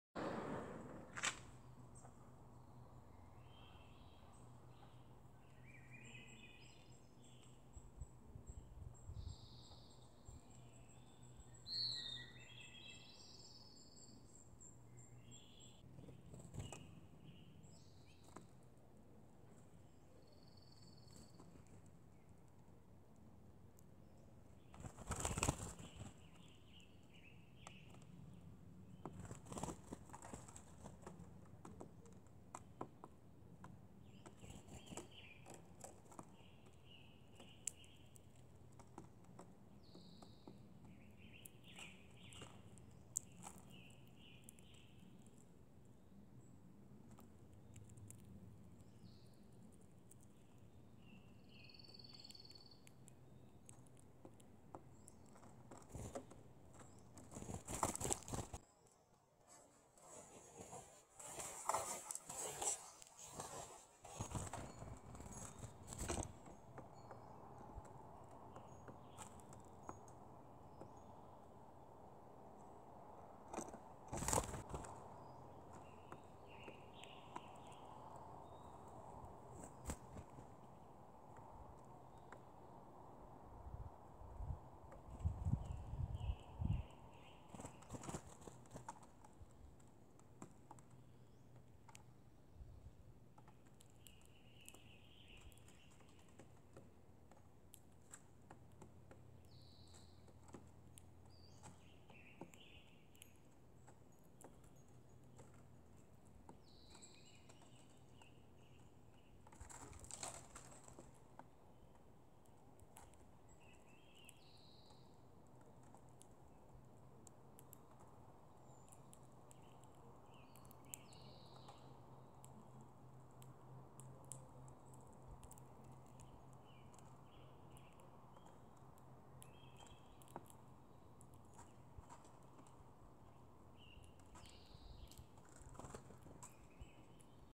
Two Birds on a Rail
A Black-Headed Grosbeak tries to intimidate a Spotted Towhee while snacking on our deck rail.